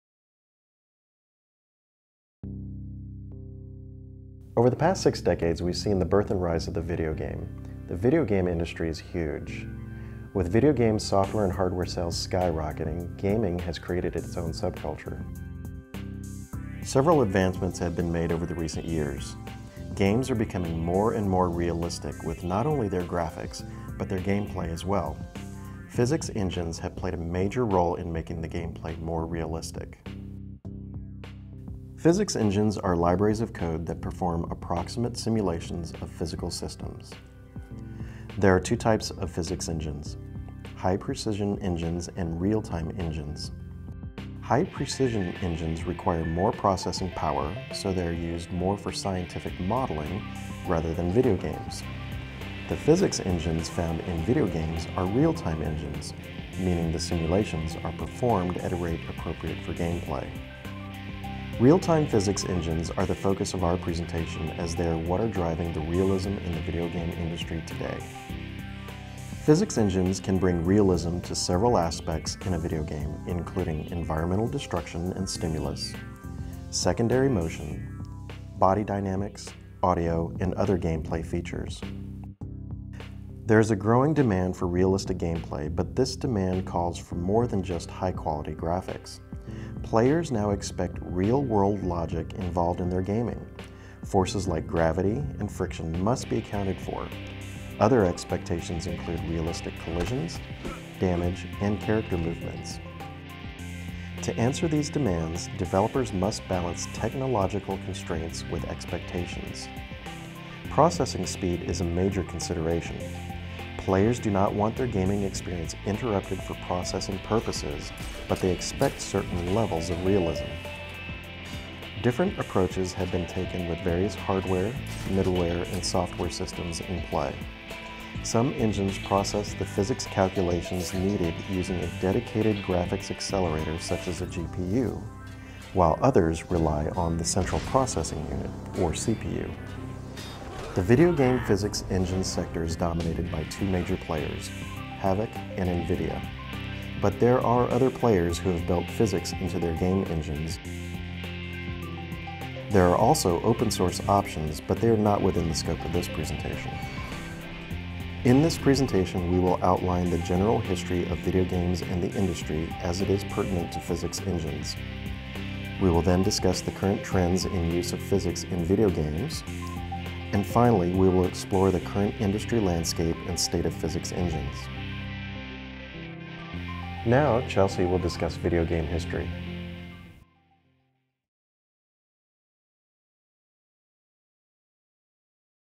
Introduction To Video Game Physics Engines
This is a video introduction for our University of Washington, MCDM Evolutions and Trends in Digital Media class - topic; video game physics engines.  I wrote the script with help from classmates.  I shot the on-screen portions - this involved lighting, audio, camera work and directing.  I edited the entire piece and mixed audio using Final Cut.  This was part of a series of three videos (about 15 minutes in TRT), which took a total of six days to produce from concept to final delivery.  Music by Brian Blake Video credits to be uploaded soon.